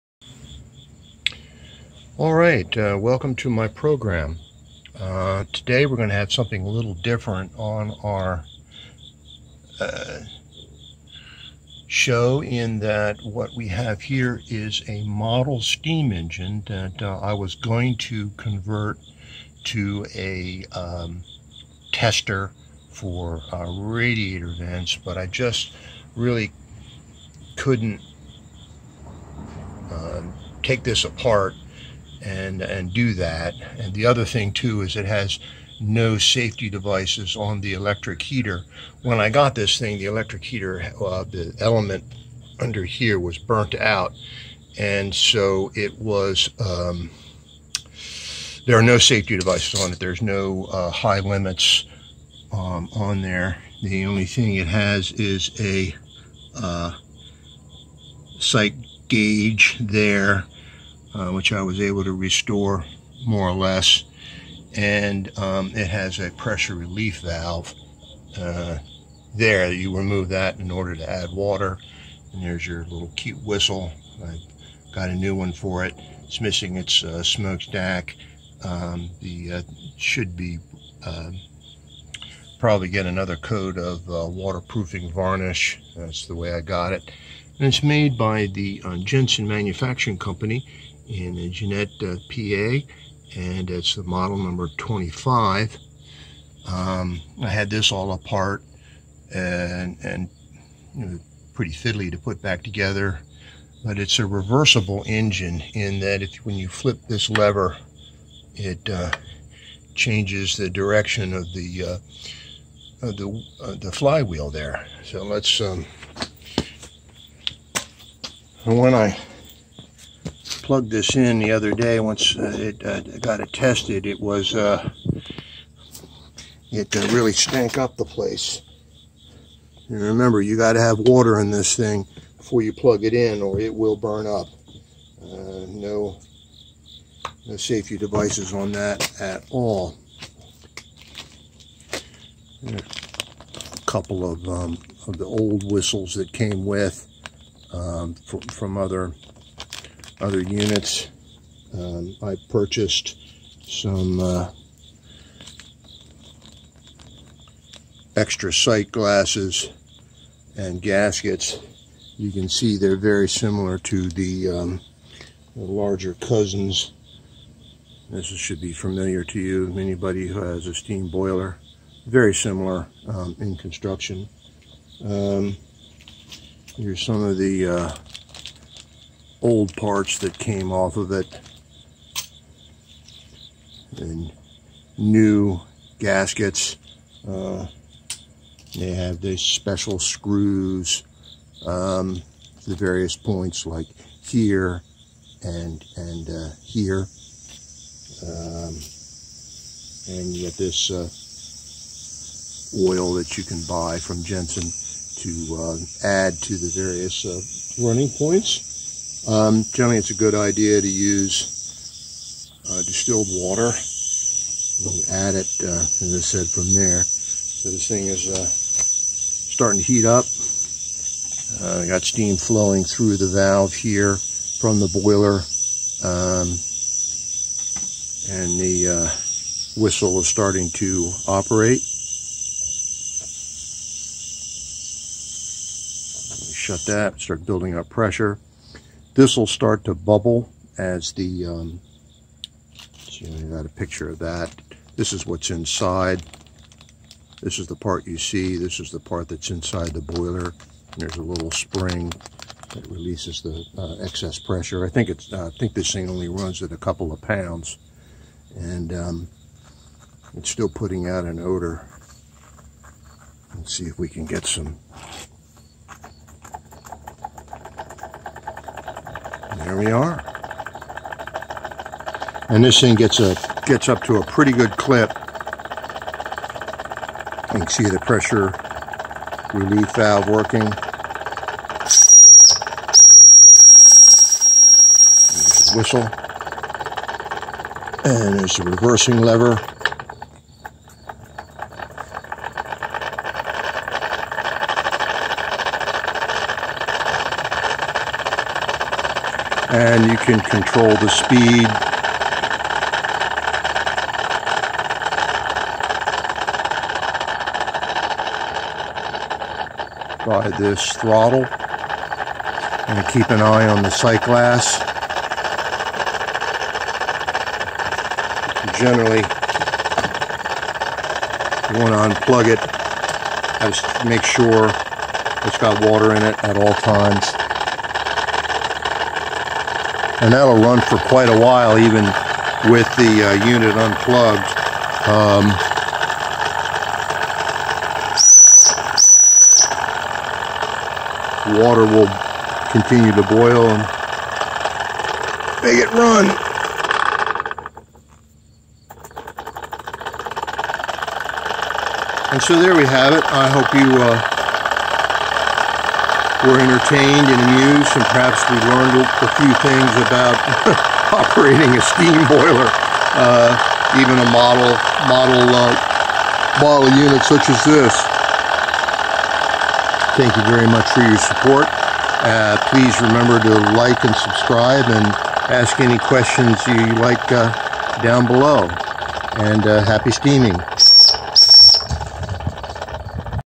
Jensen Model #25 Toy Steam Engine Partially Restored But Now Fully Functional
I bought this unit to take apart to make it into a portable steam heating radiator vent tester, but
I just could not bring myself to do that to this nicely crafted model steam engine by Jensen.
Also, I would have needed to add some kind of burn-out/temperature safety control on the heater coil for my own piece of mind before taking this out in the field.
A pressure gauge would be nice, too.
I bought an after-market adapter to add a pressure gauge, but I would have had to give up the whistle.
No way!